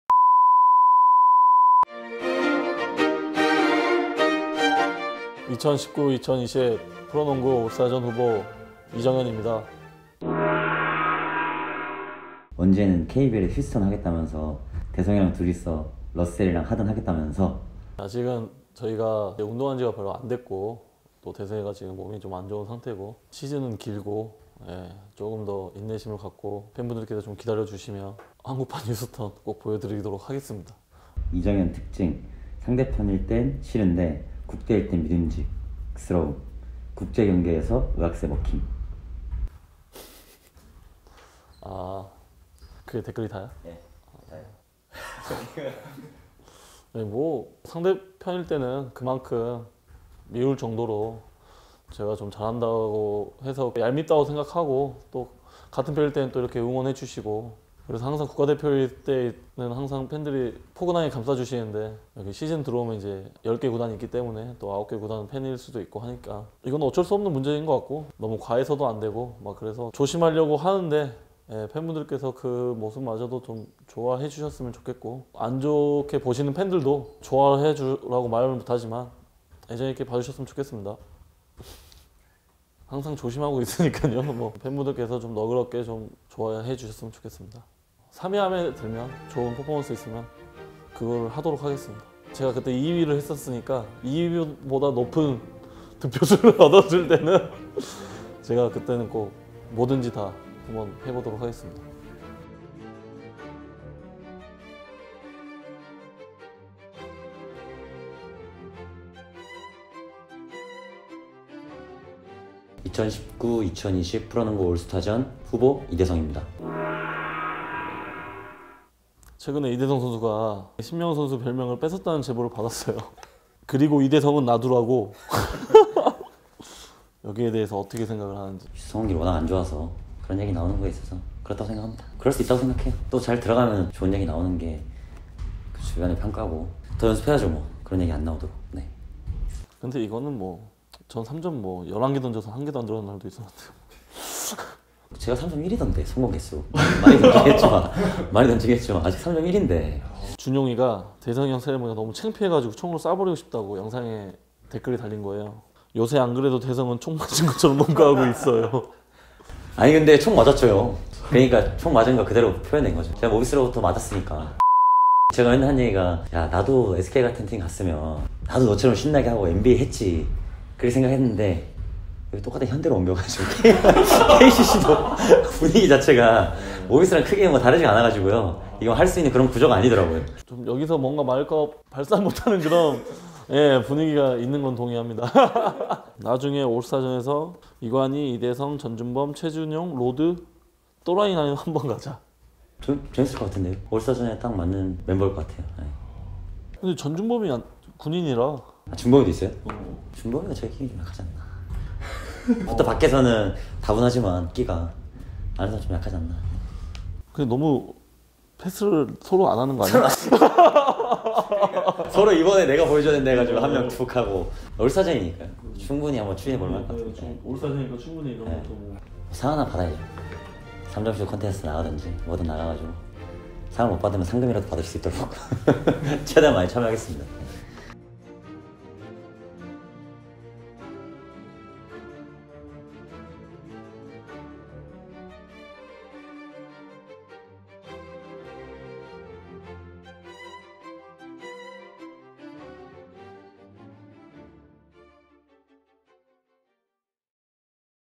'댓글 부자' 이정현-이대성의 댓글 읽기
올스타 후보 검증의 시간 - KCC편

댓글 부자 이정현, 이대성의
올스타 공약은?!